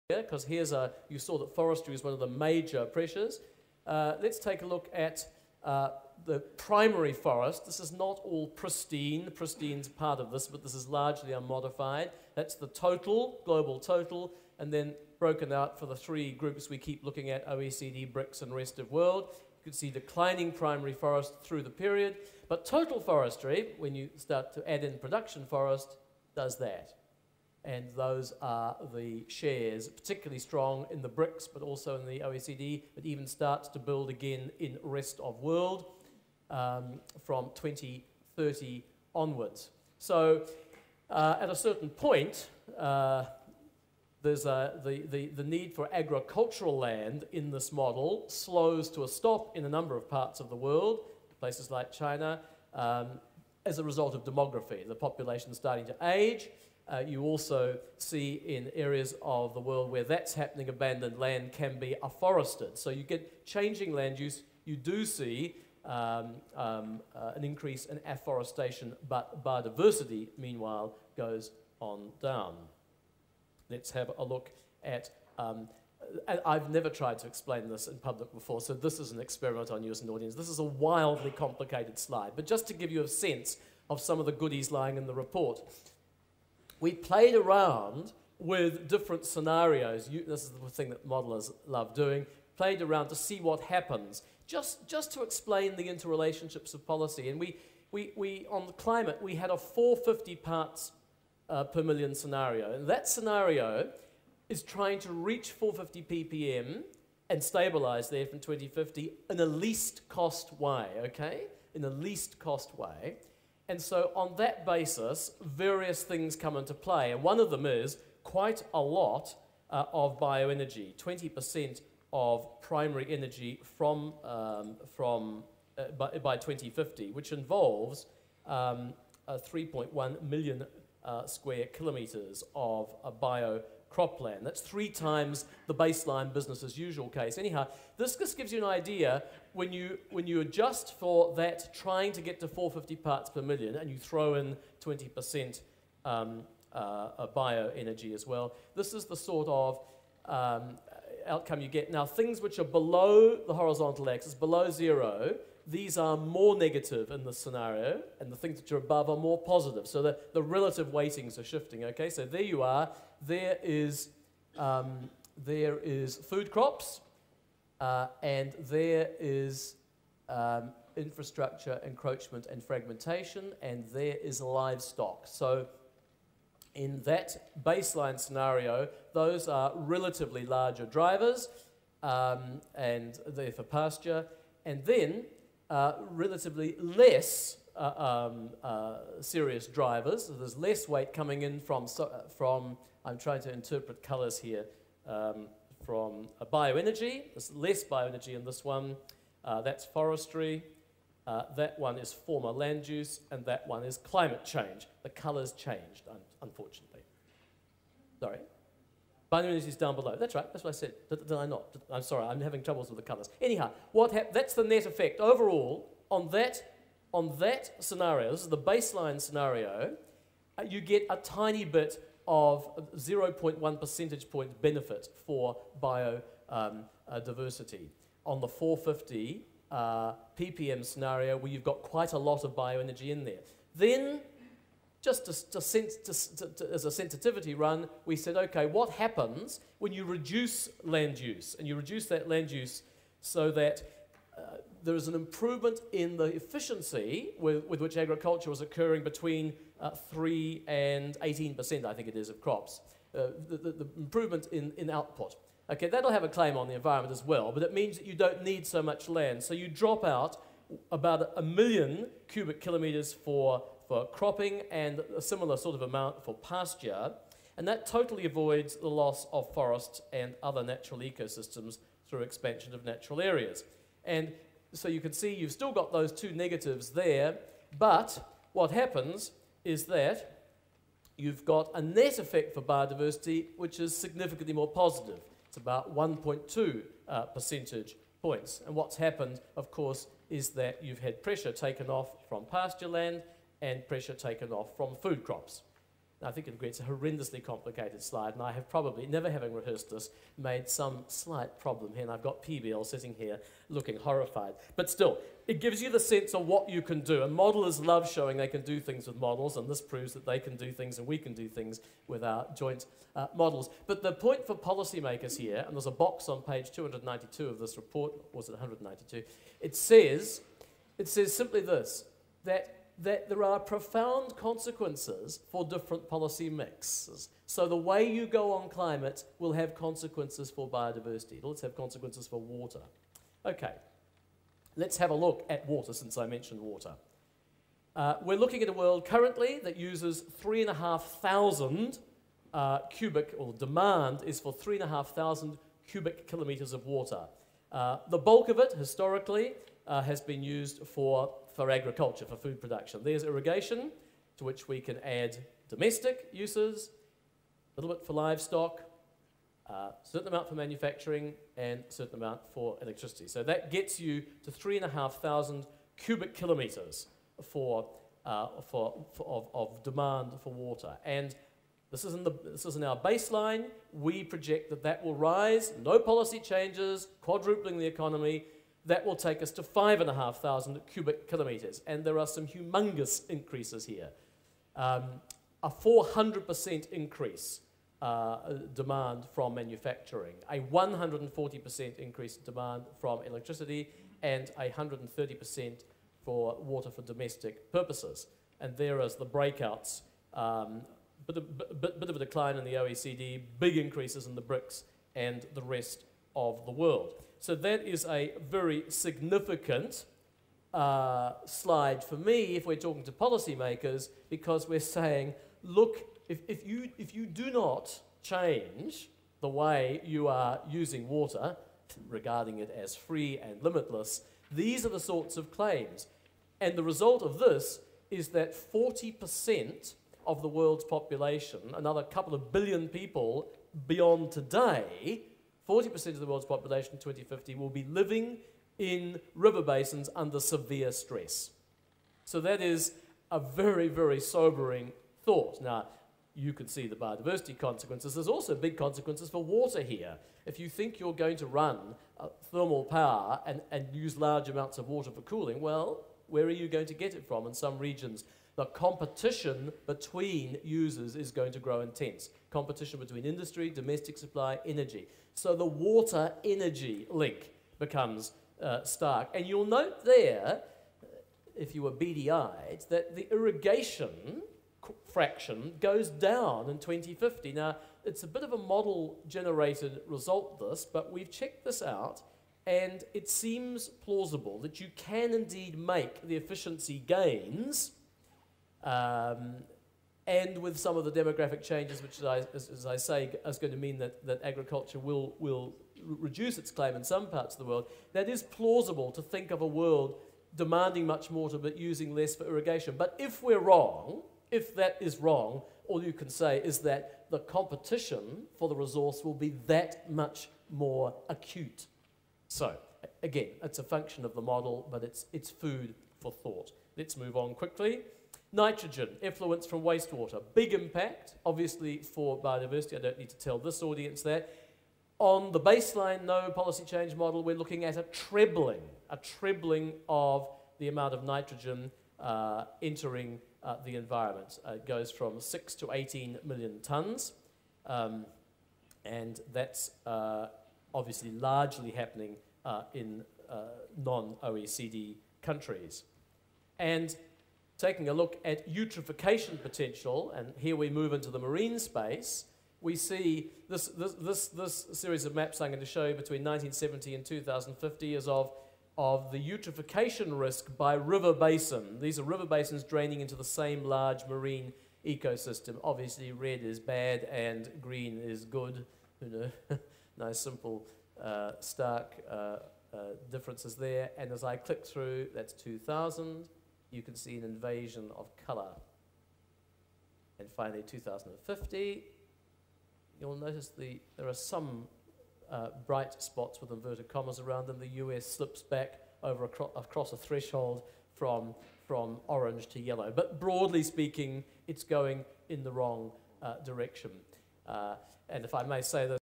Plenary1-(4of9)-welcome-adress-panel-discussion
Welcome address: The Challenges for Science and Implementation
of Mainstreaming TEEB and panel discussion